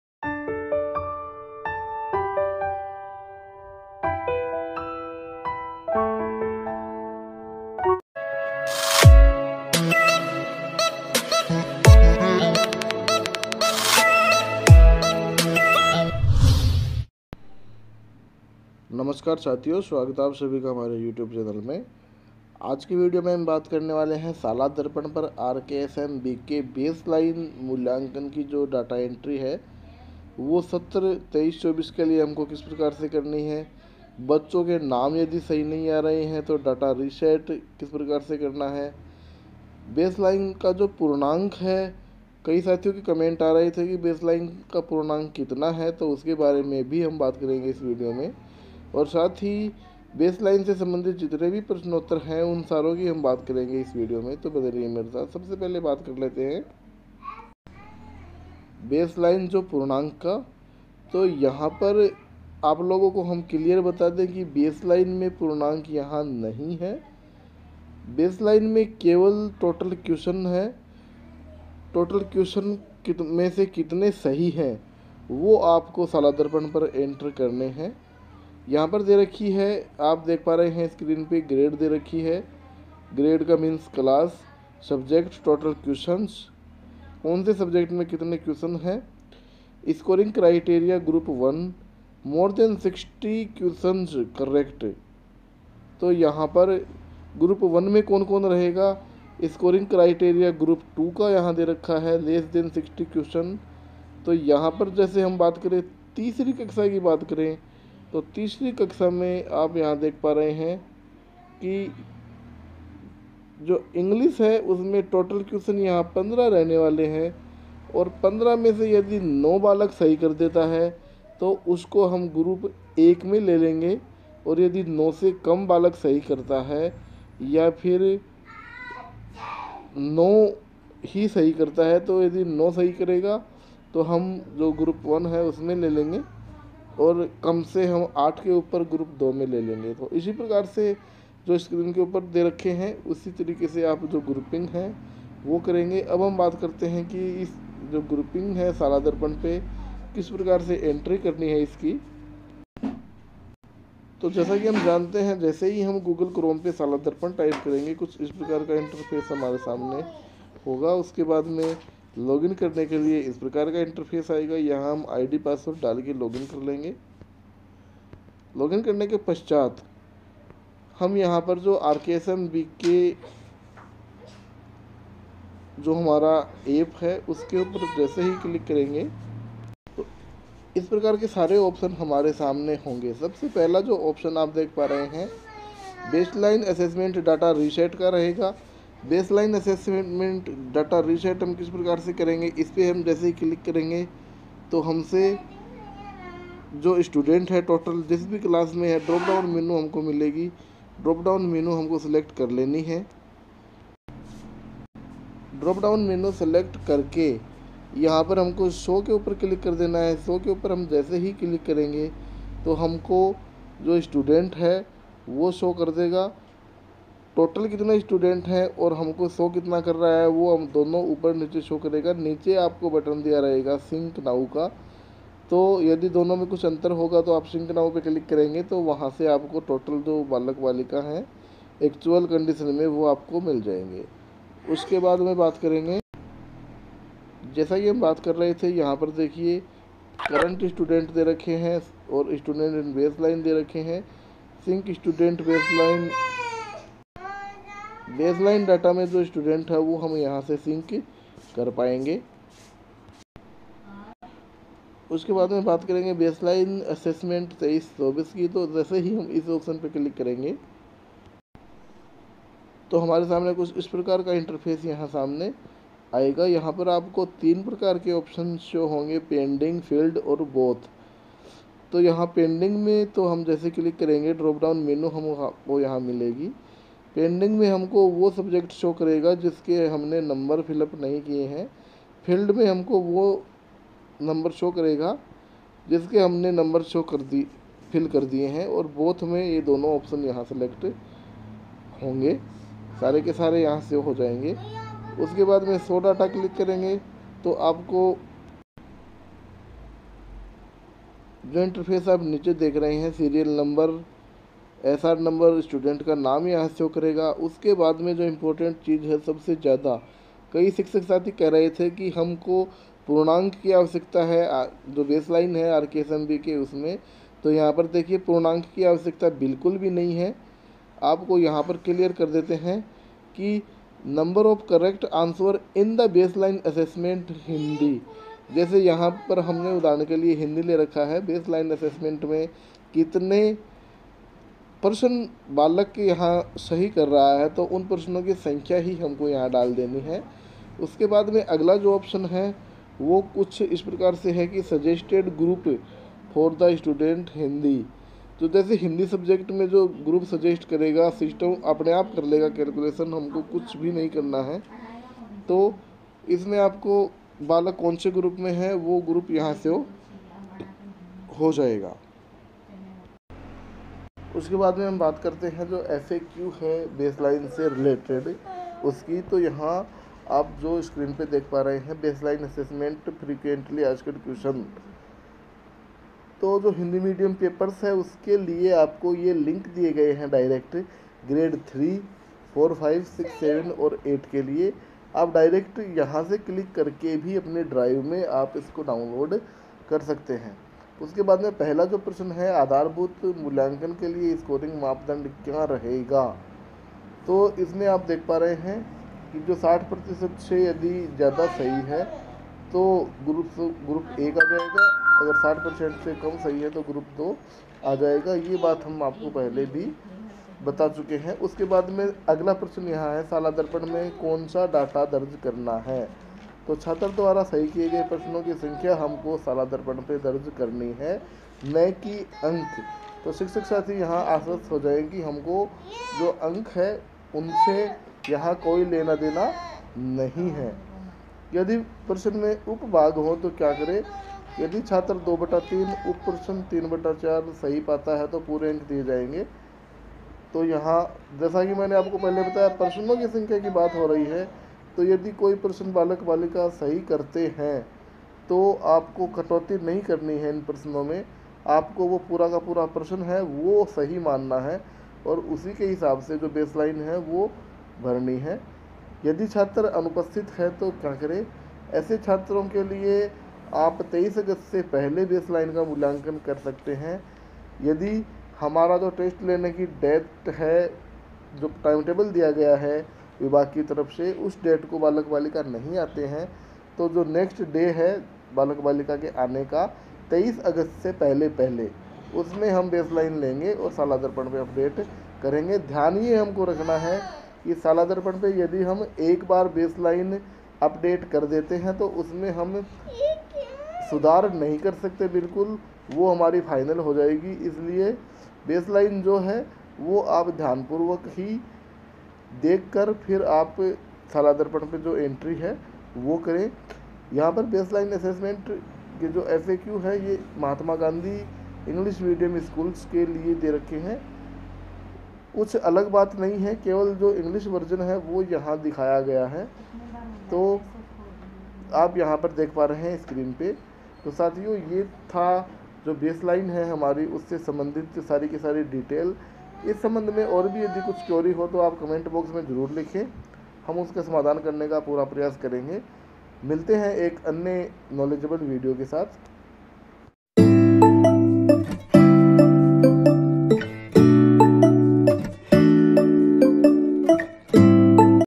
RKSMBK बेसलाइन टेस्ट समस्याहेतु FAQ जानें पेपर में अंक हैं या नहीं,बेसलाइन ऑनलाइन एंट्री कैसें करें
1-पेपर के सही लिंक कोनसे है
2-पेपर में अंक हैं या नहीं
3-समुह 1या दो 2 कैसे बनाएं
4-पूरा प्रश्न सही न हो तो क्या करें
5-छात्र अनुपस्थिति हो तो क्या करें
6-शालादर्पण पर कोनसे डेटा की एंट्री होगी
7-अंतिम तिथि क्या है

🏛️ Directorate of Secondary Education - Bikaner

🔹📖 RKSMBK बेसलाइन असेसमेंट - सभी पीईईओ और संस्था प्रधान को इन बेसलाइन असेसमेंट को मुद्रित करना छात्रों को वितरित करना होगा। 🔴📕

Baseline Assessment Dates: 16th - 18th August 2023

अंक प्रविष्टि - बेसलाइन मूल्यांकन अंक शाला दर्पण पर दर्ज किए जाने हैं। डाटा दर्ज करने की अंतिम तिथि 23 अगस्त

🔹📖 RKSMBK English Medium papers - बेसलाइन असेसमेंट - सभी पीईईओ और संस्था प्रधान को इन बेसलाइन असेसमेंट को मुद्रित करना छात्रों को वितरित करना होगा। 🔴📕

Hindi Medium papers

अंक प्रविष्टि - बेसलाइन मूल्यांकन अंक शाला दर्पण पर दर्ज किए जाने हैं। डाटा दर्ज करने की अंतिम तिथि 23 अगस्त है